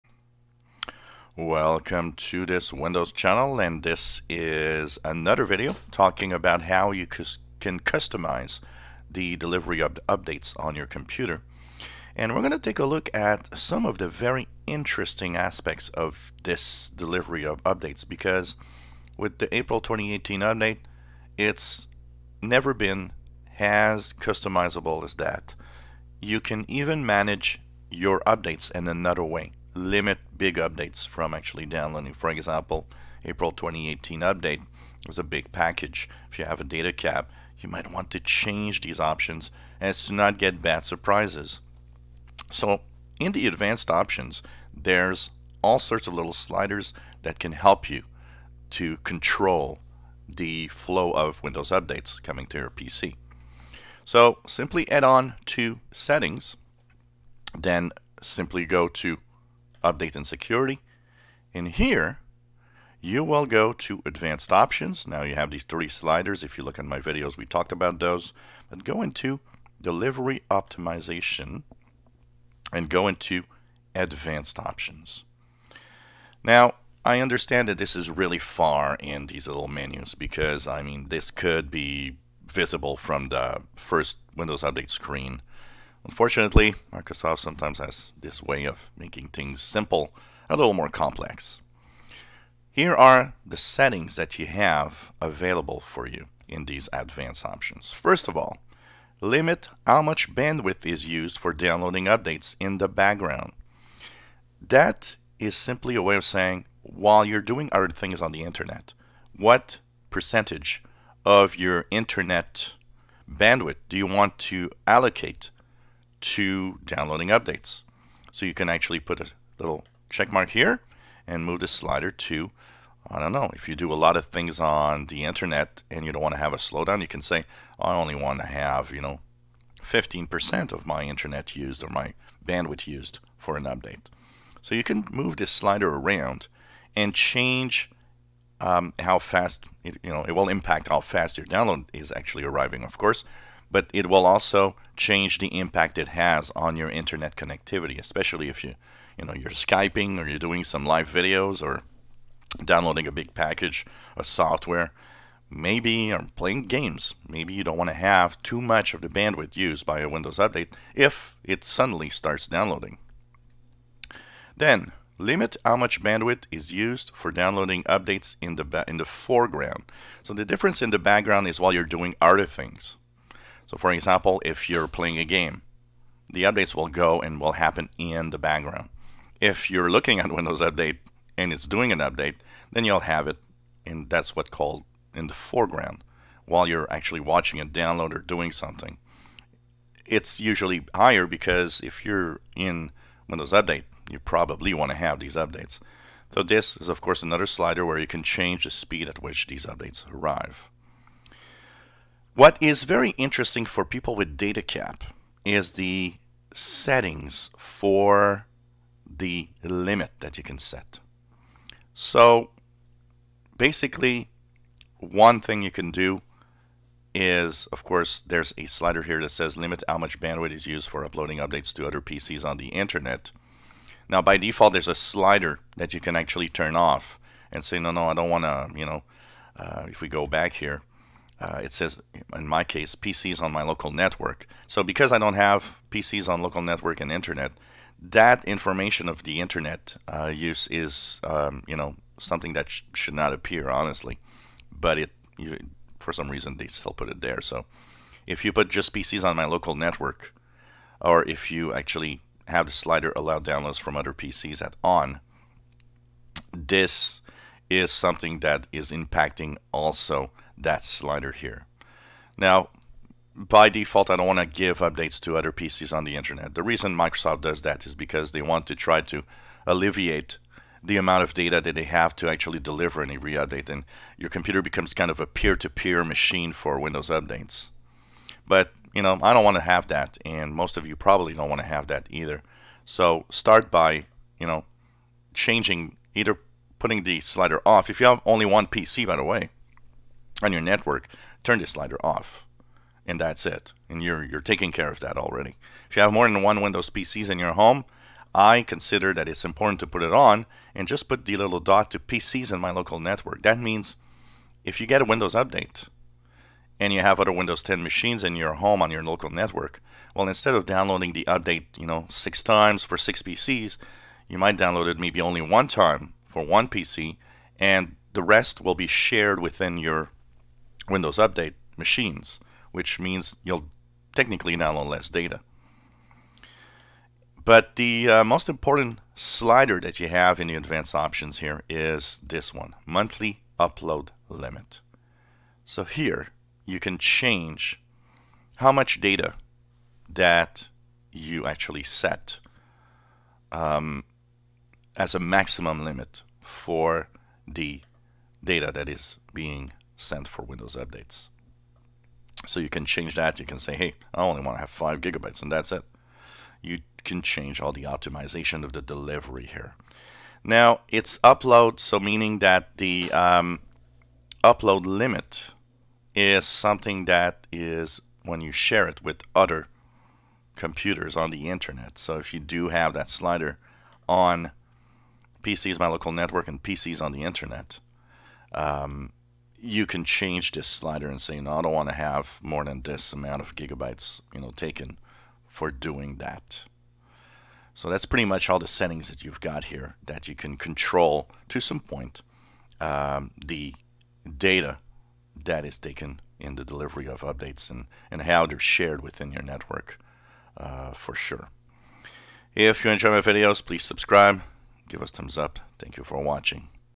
Windows 10 April 2018 update Delivery optomization of Windows updates
Checking advanced options in the Delivery optimization of Windows updates